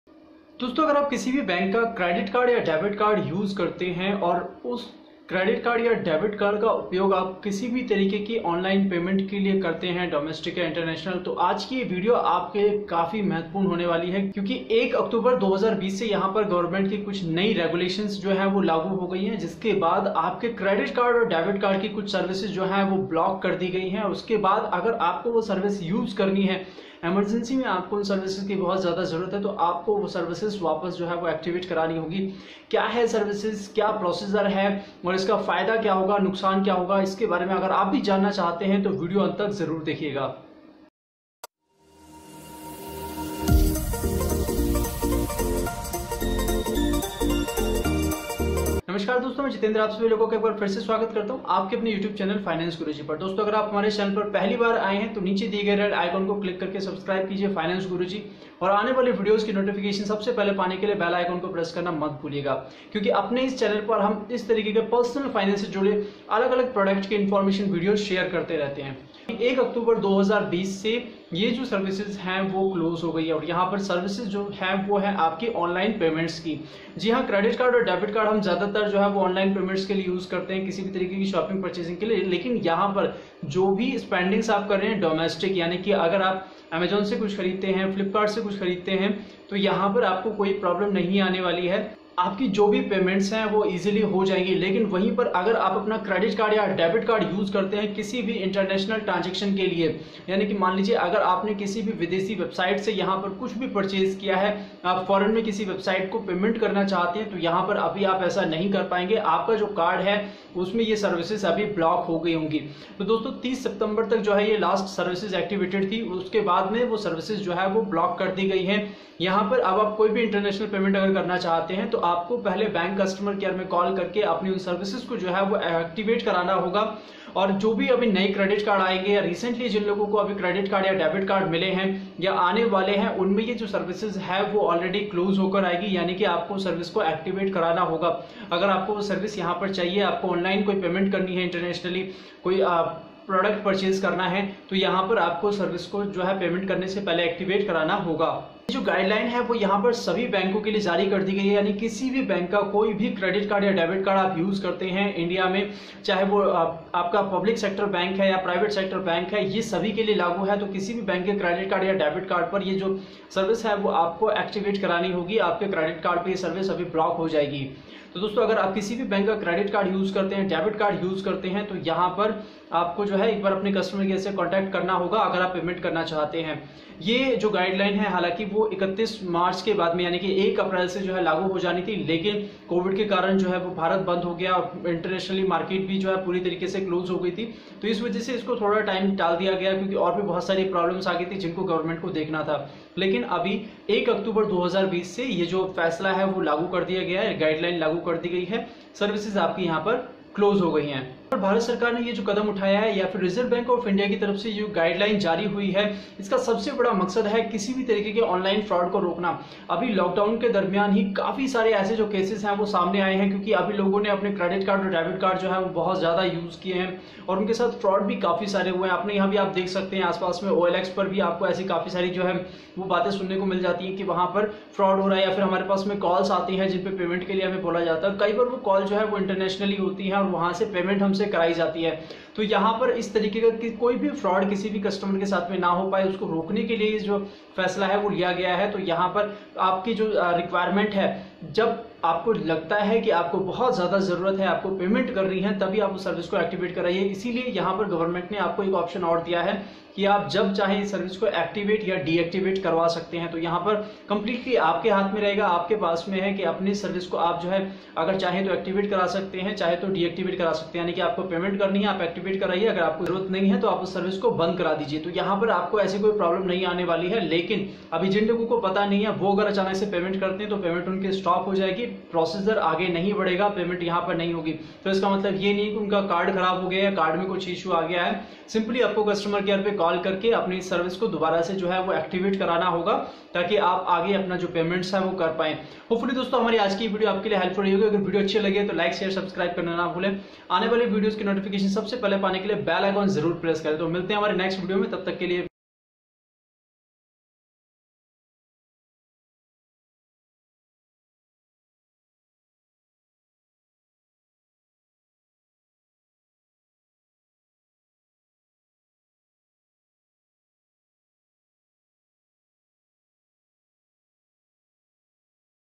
आपके Credit Card & Debit Card Update मे बंद हुई ये सुविधा
Credit Cards & Debit Cards New Updates | Online Payment Rule Change | आपके क्रेडिट और डेबिट कार्ड मे हुआ है बड़ा बदलाव |
Friends from 1st October 2020 Credit Card and Debit Card online payments for International transactions have been stopped. Now Everyone who wants to make international payments or wants to use his/her card out of the country has to ask the bank to activate international payment services.

[ Follow us on other social media accounts ]

[ Read On Blogs ]